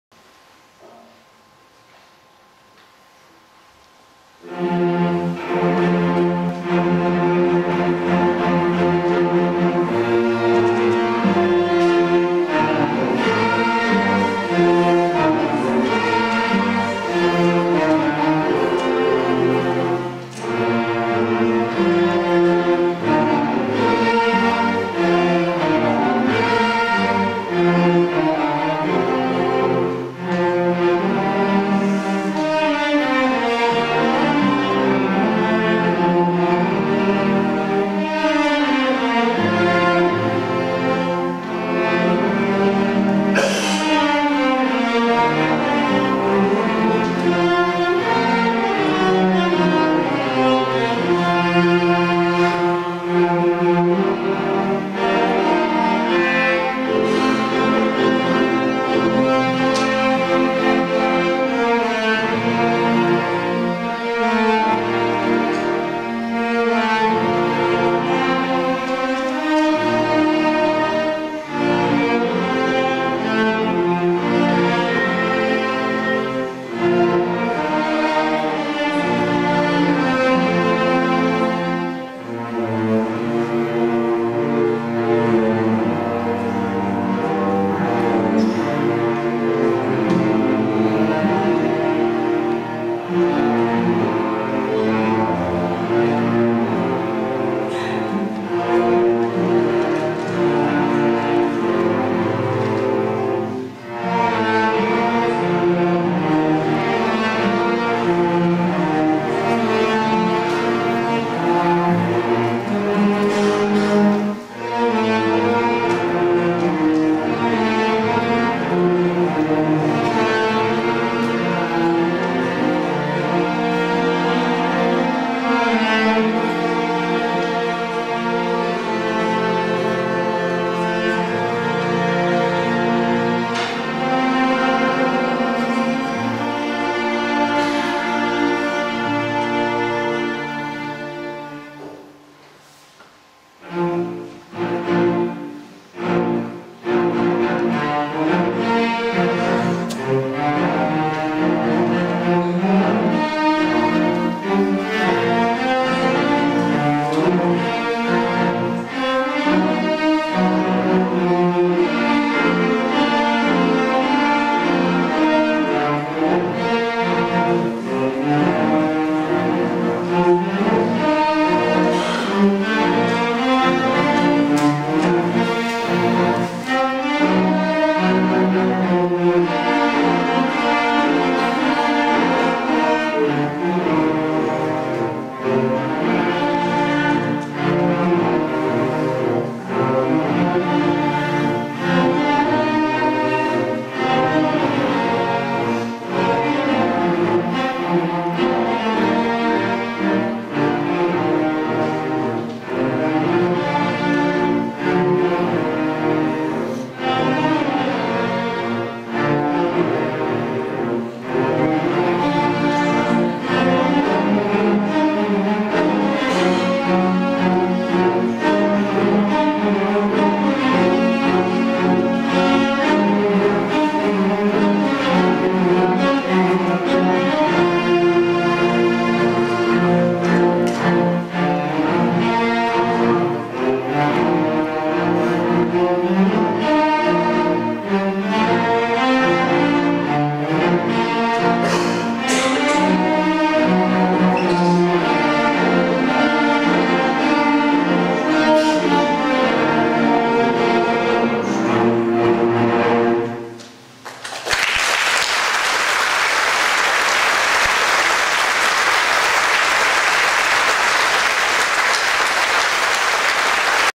Lower Strings - Themes from the Movies
Lower Strings - Themes from the Movies - Knox Academy Christmas Concert 2012